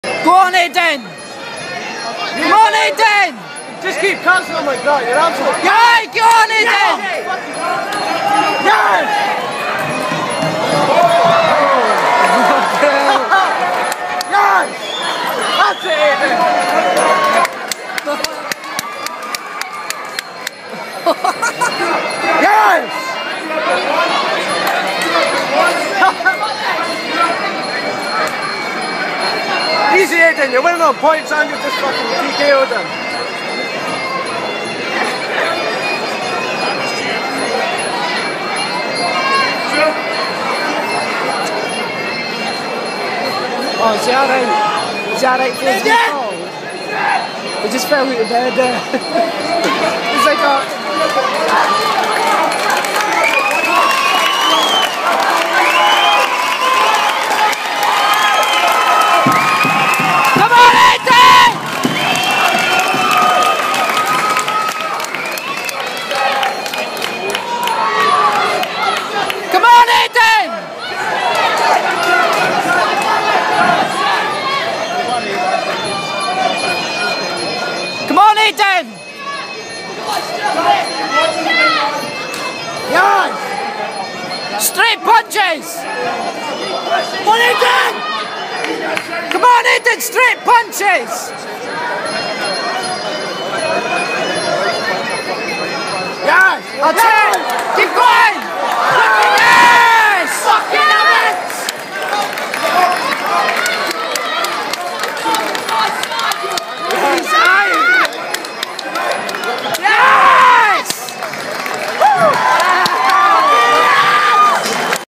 Aidan Stephen (Moray Muay Thai) Vs Stewart McCarroll (Rama Muay Thai) Round 4
Round 4 of Aidan Stephen of Moray Muay Thai vs William Stewart McCarroll of Rama Mauy Thai at MTB 4 in Johstone 25/08/12